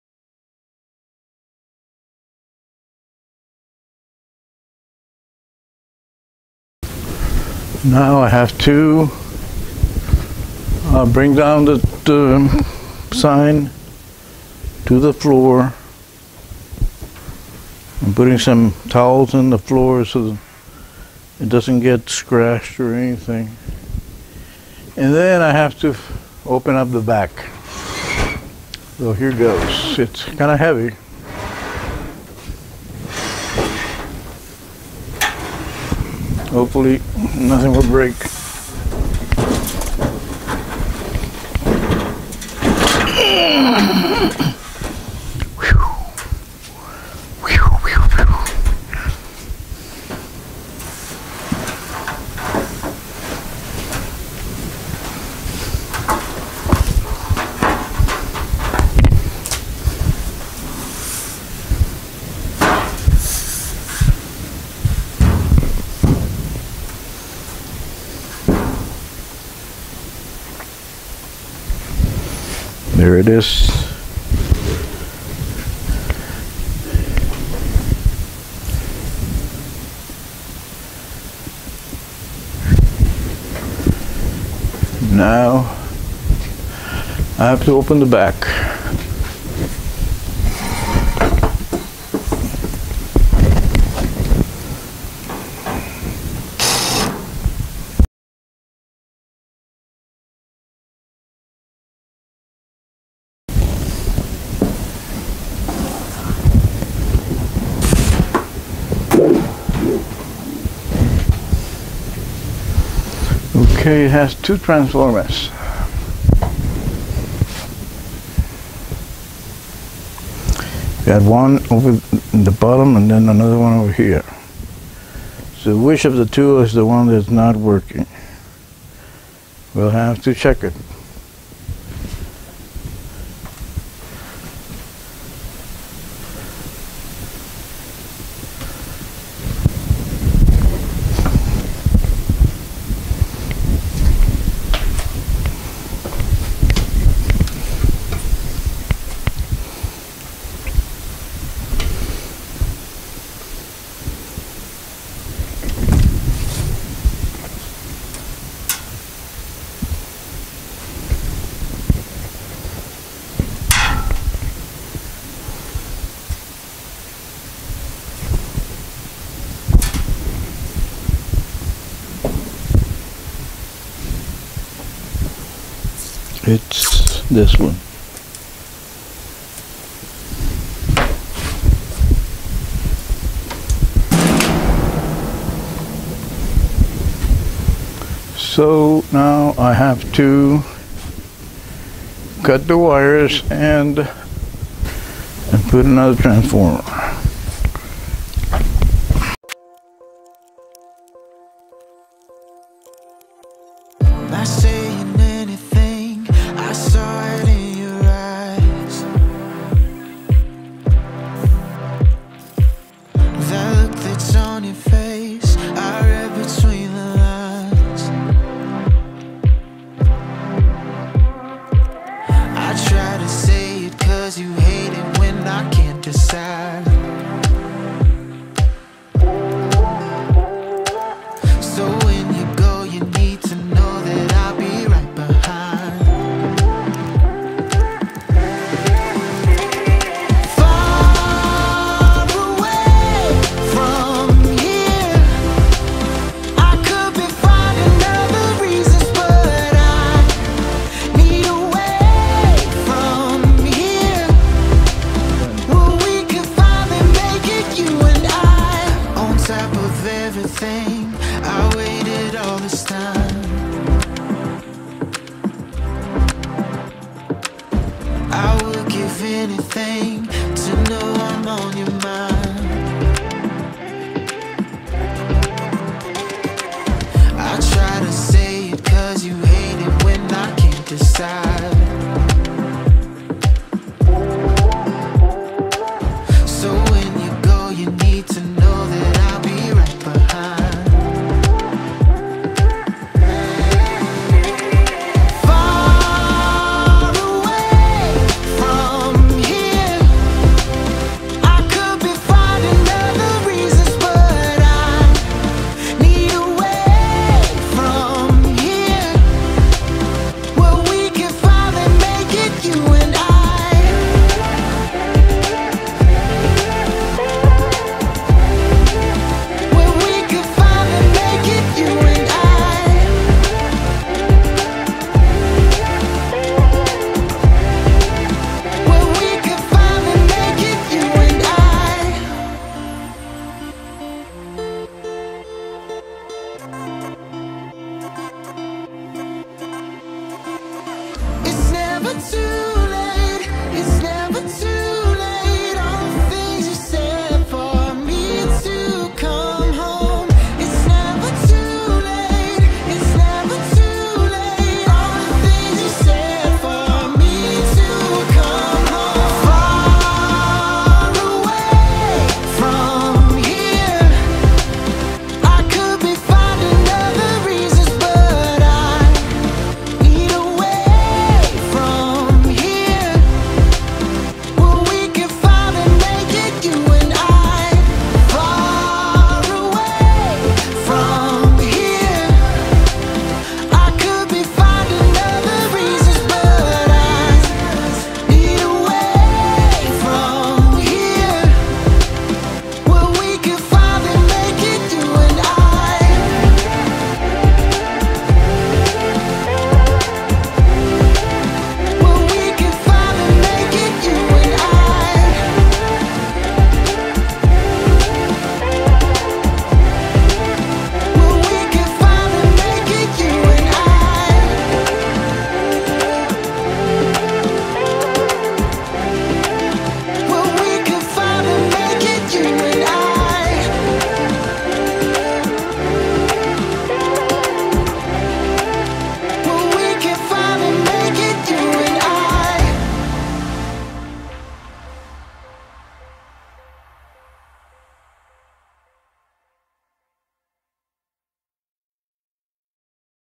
Replacing a Transformer On A Chevrolet Neon Sign
Video of replacing a transformer on a Chevrolet Neon SIgn and the delivery to the Mancave.